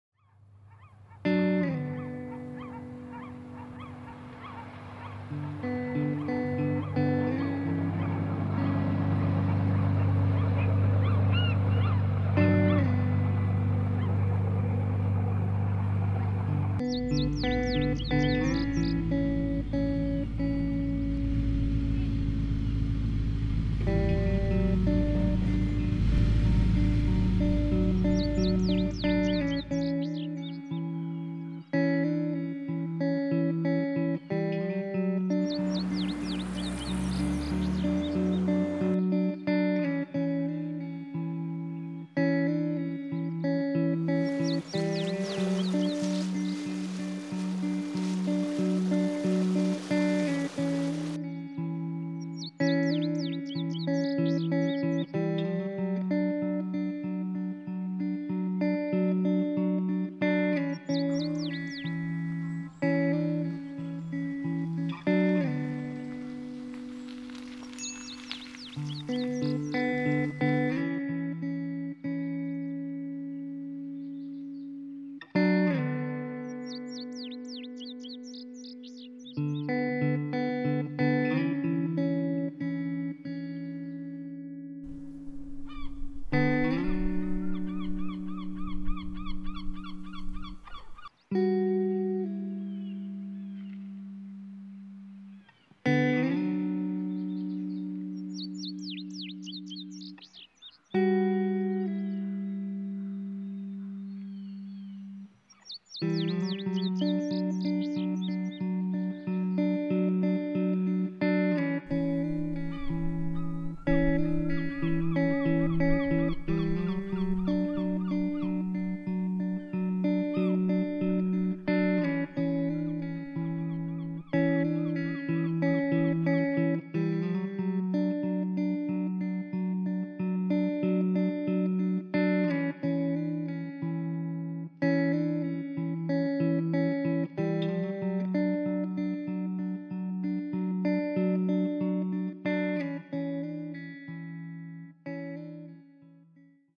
Printemps en Corbières
Découvrez les premières couleurs des Corbières, de la mer à la terre, à la sortie de l'hivers. Un film 100% drone.